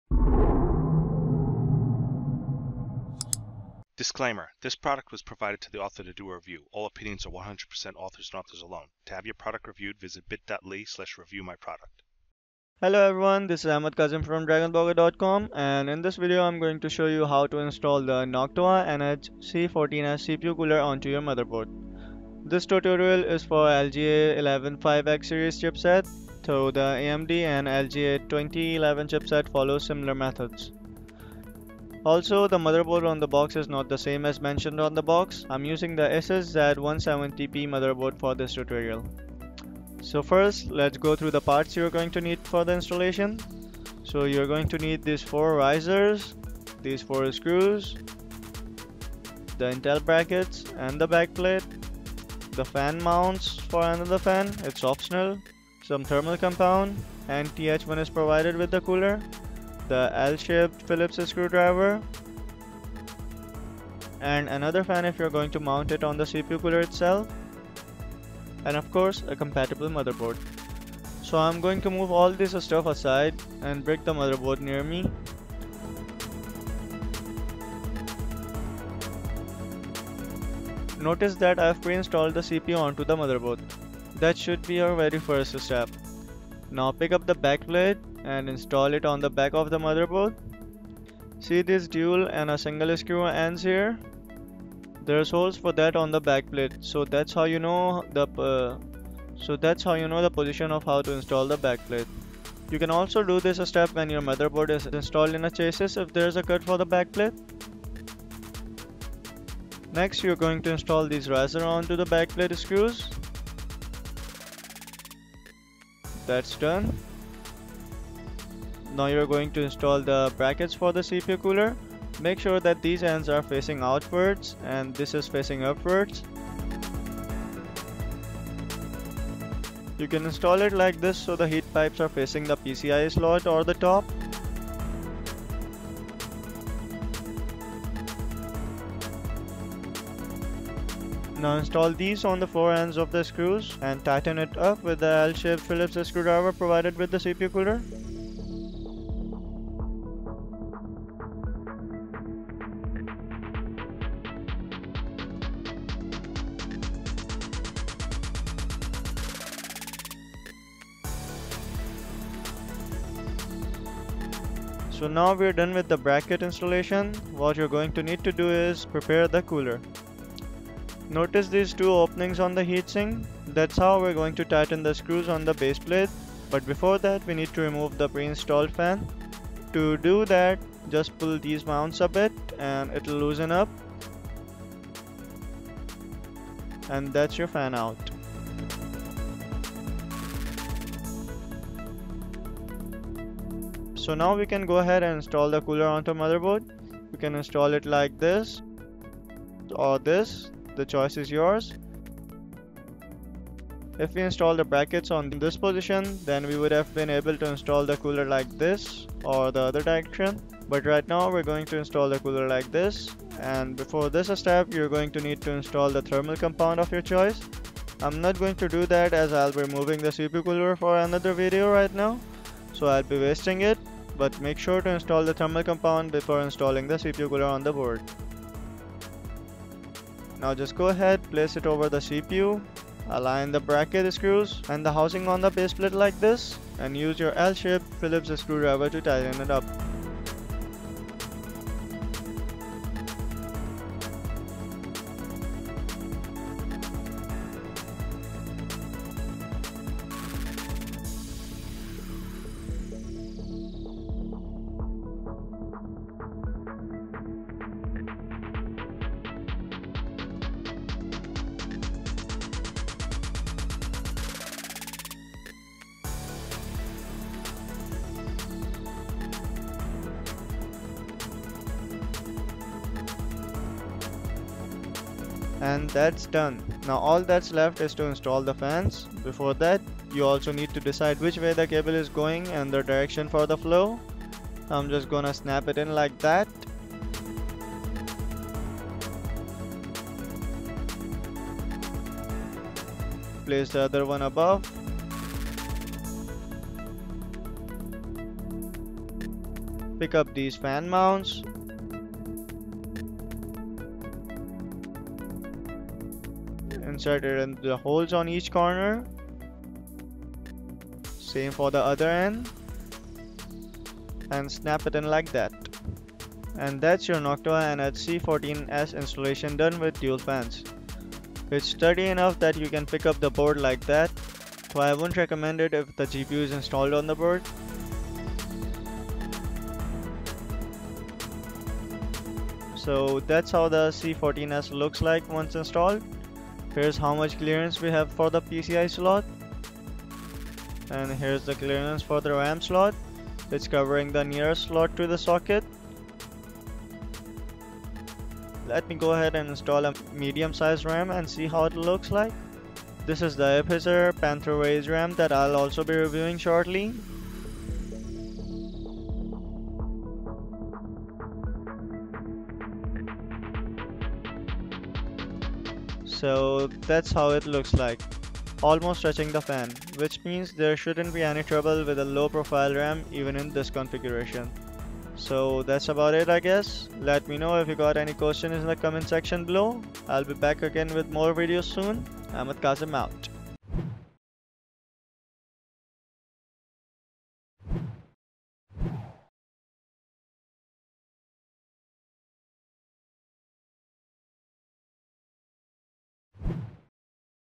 How To Install Noctua NH-C14S With Dual NF-A14 Fans
In the following video, I show you how to install the Noctua NH-C14S onto the Asus Z170P motherboard.

All the necessary components are provided by with the Noctua NH-C14S.